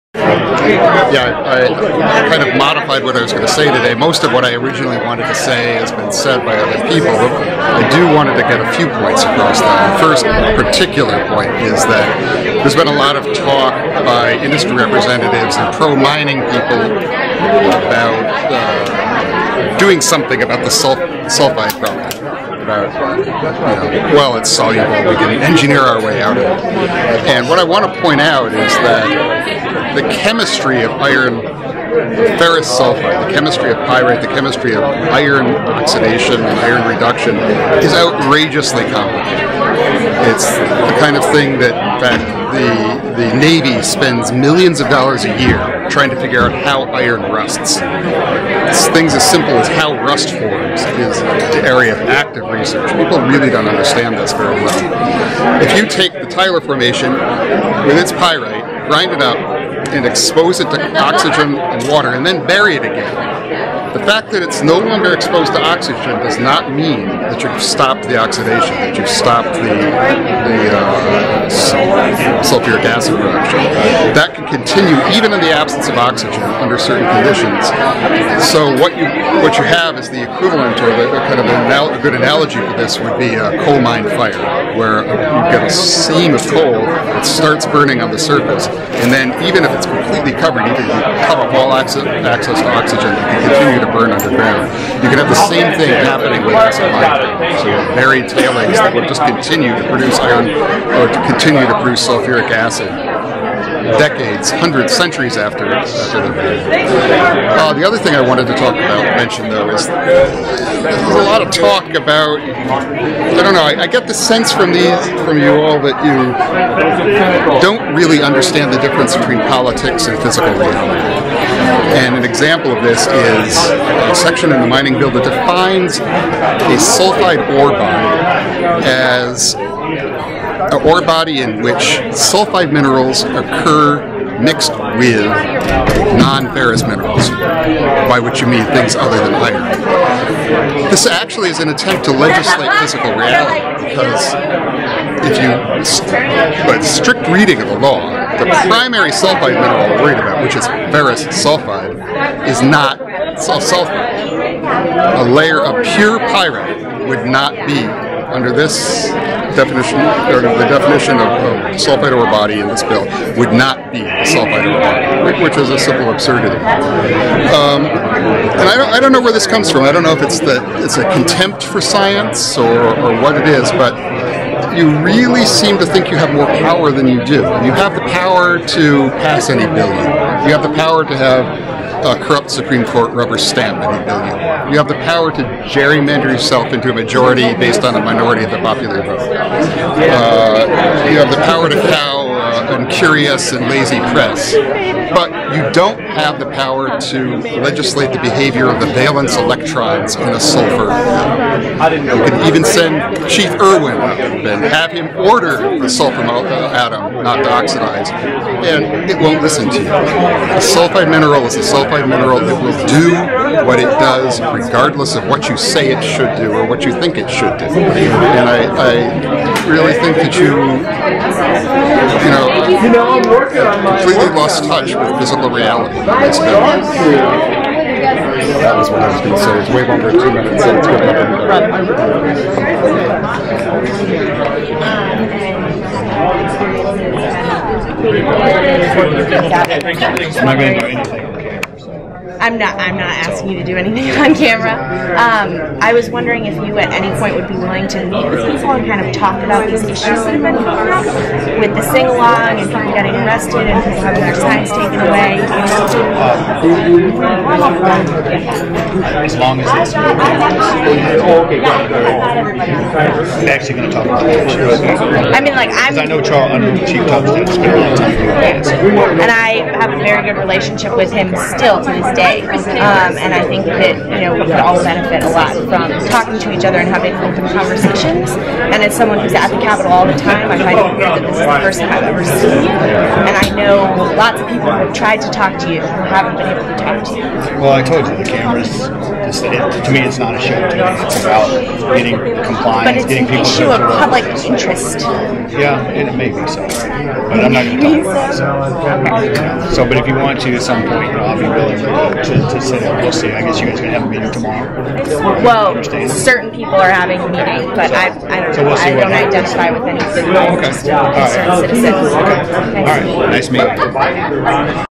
Public Hearing Shut Down... Scientist: "The Legislature Has Lost Touch with Physical Reality"
"You don't have the power to legislate the behavior of the valence electrons of a sulfur atom. You can send Chief Erwin up there and have him ORDER the sulfur out of the oxides, and it won't listen to you."

1.23.13  Geochemist Joseph Skulan waited ten hours Wednesday to testify at the only scheduled public hearing on the GTac mining bill, and in the end was barred from speaking. The hearing was shut down at 9 p.m., before many citizens, some of whom had traveled hundreds of miles, had had the opportunity to have their voices heard. As the crowd in the hearing room was dispersing, Skulan gave his testimony in front of a camera, with Capitol Police Chief David Erwin looking on.

As Skulan continued his testimony, Erwin moved across the room and was approached by a young woman. His initial comment to her was, "I'm not going to do anything on camera." The woman asked Erwin if he'd be willing to meet with people to talk about issues with the [Solidarity] Sing Along and the arrests that have taken place in the Capitol. His response was, "As long as it's no cameras. "